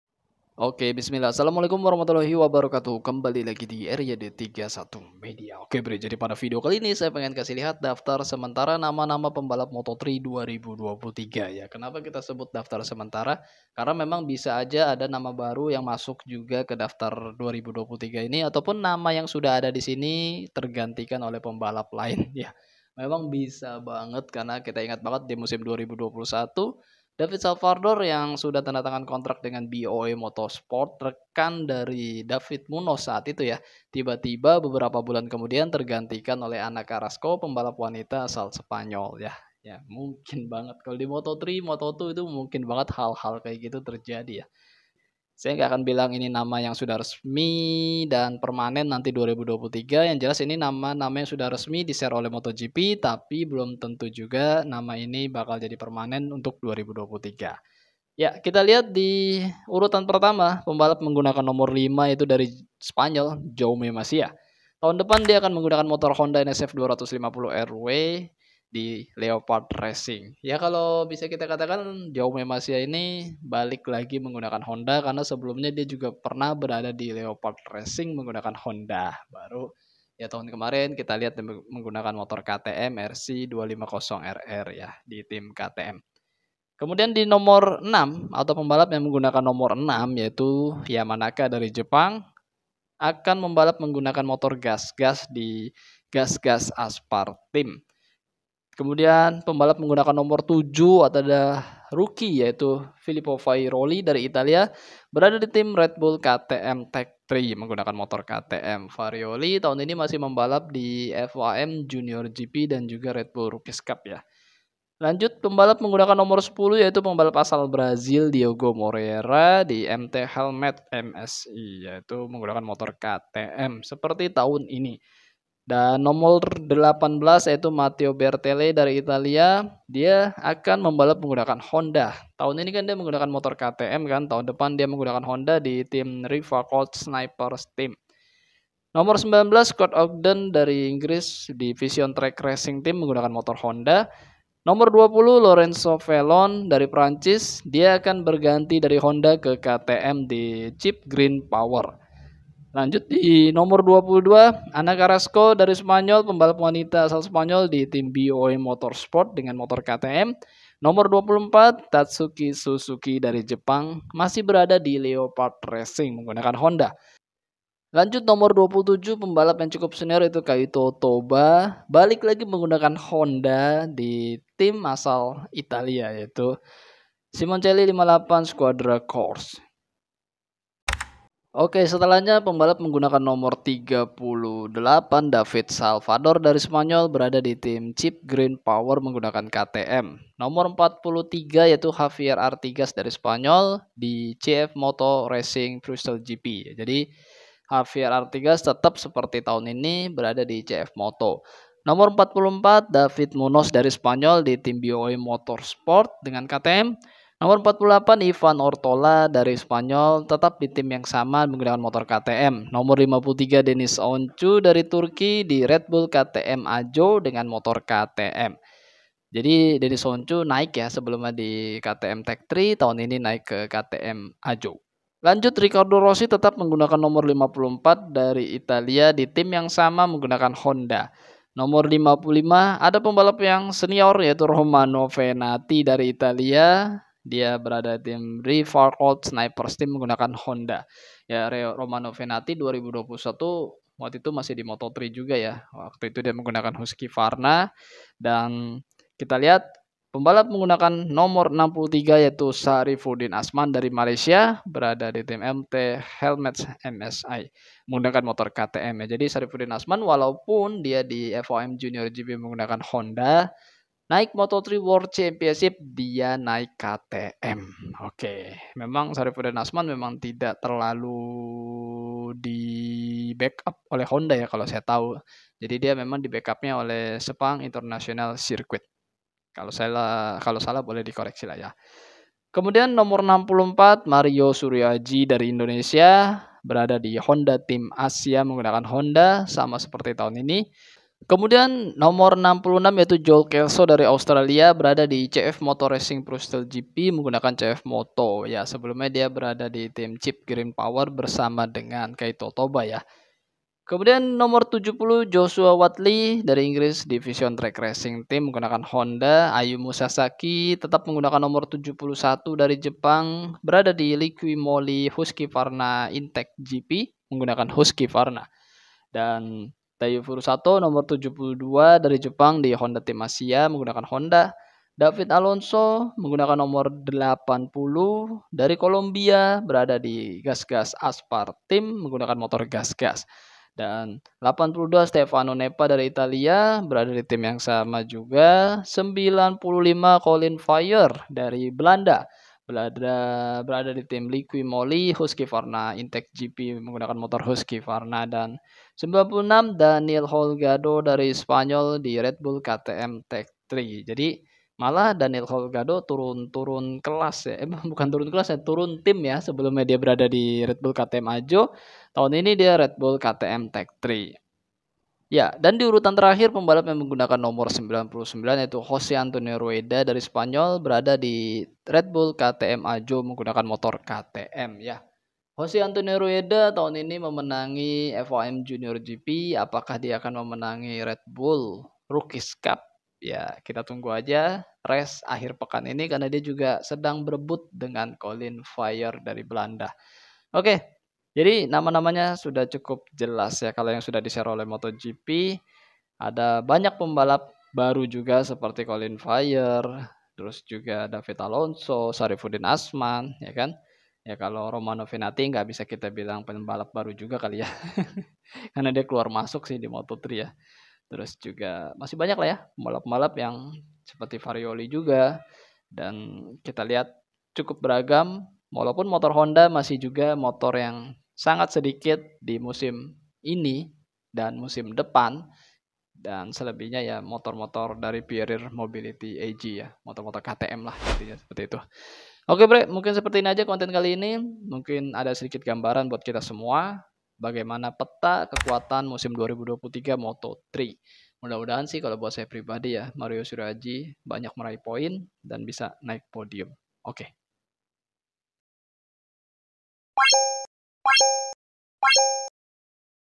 Daftar Nama Pembalap Moto3 Musim 2023
Mario Suryo Aji jadi wakil Indonesia di Kelas Moto3 musim 2023
Romano Fenati balik Moto3 musim 2023